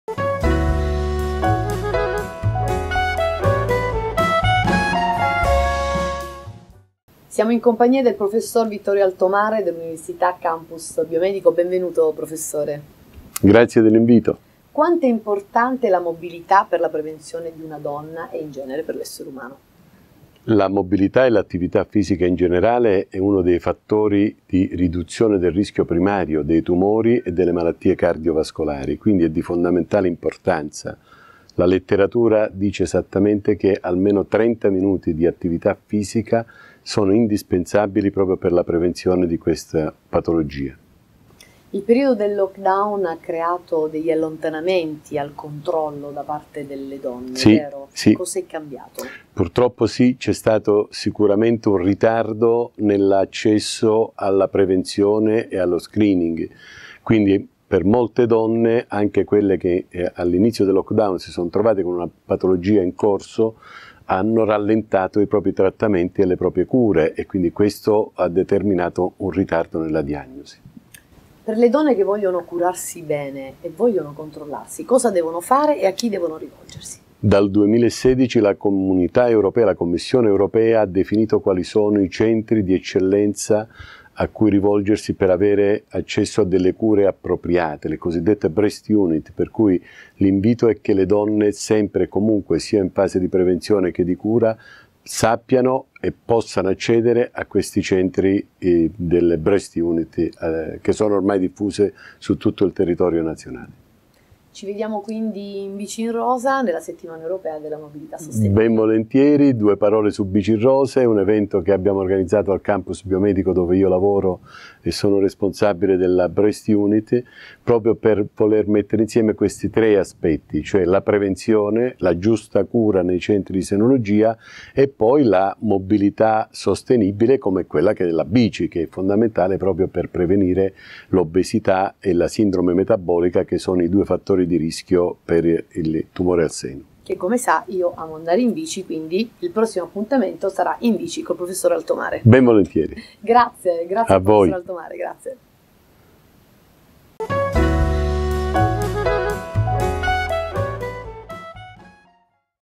La prevenzione dei tumori al seno passa attraverso la bici e lo sport
Catia Acquesta intervista il Prof. Altomare del Campus Biomedico.